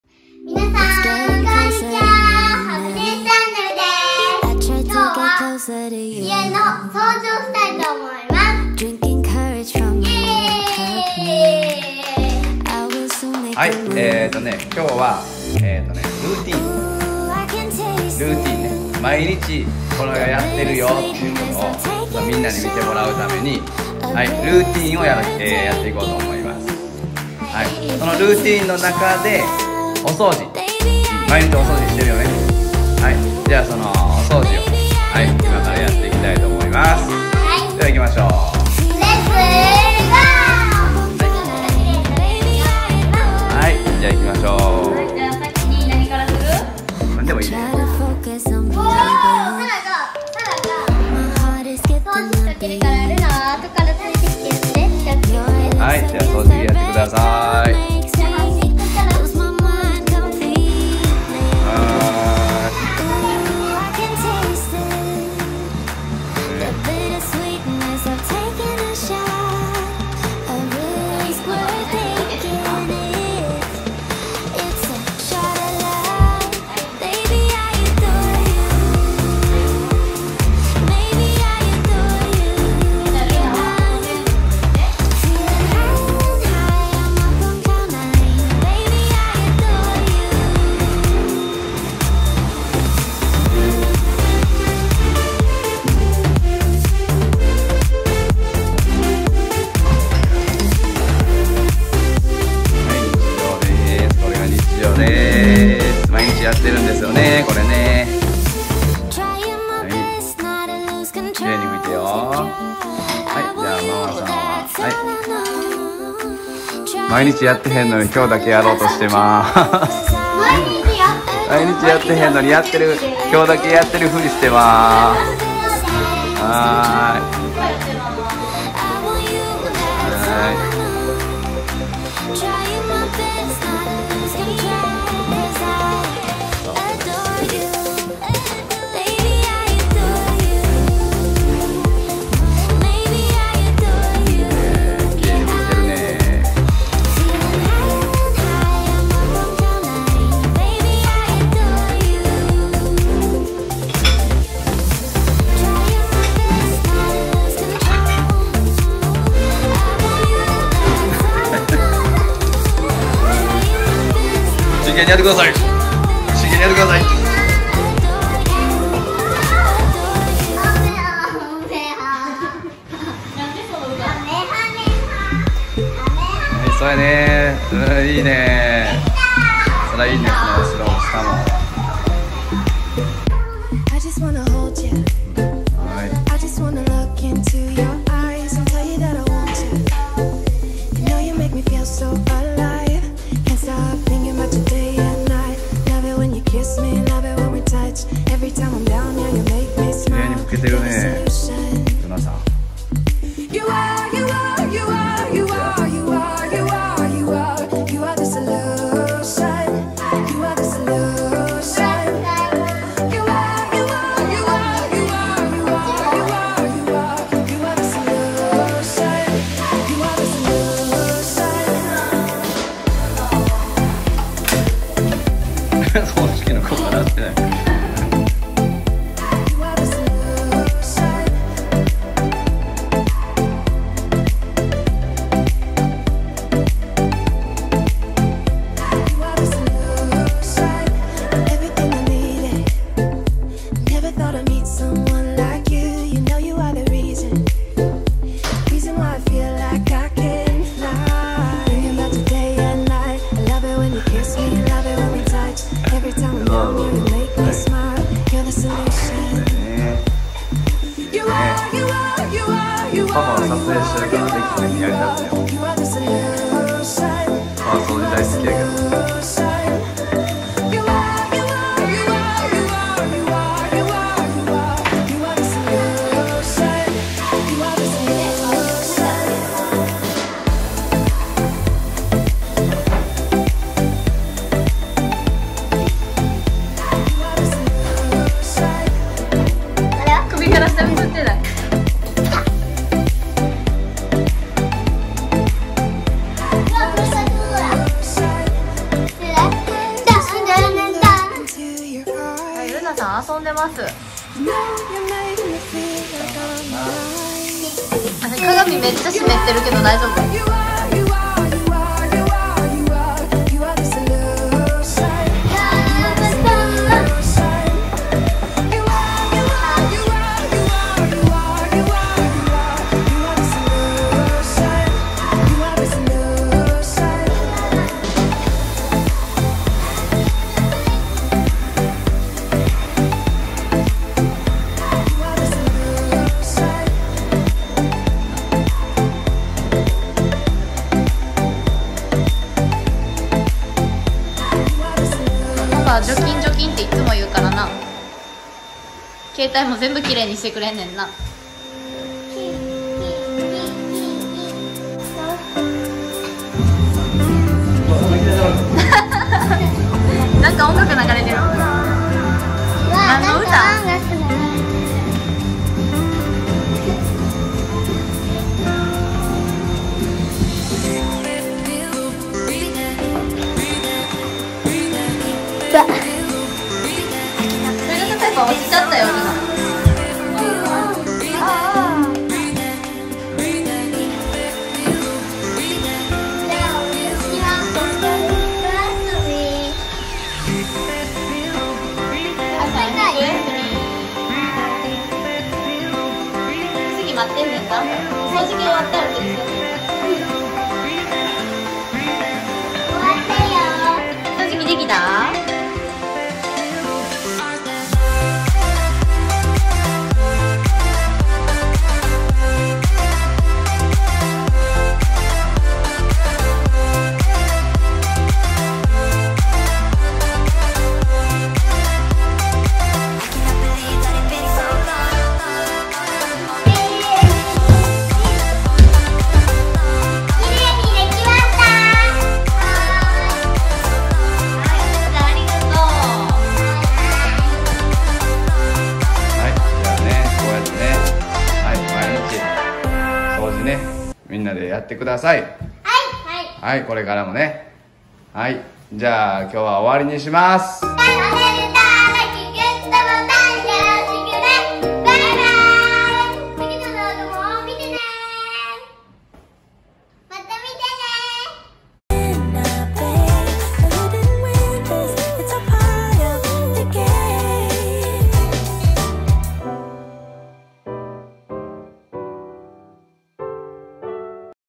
【ルーティン】ステイホーム ルームツアー新築一戸建て 休校中のリアルな過ごし方紹介 掃除編 マイホーム 注文住宅
初のルーティン動画です★
今回は、毎日のお掃除!!
休校中、外出自粛の為、お家のお手伝いを頑張ってもらっています(^^)
楽しみながら出来る掃除の仕方があれば、是非教えてください♪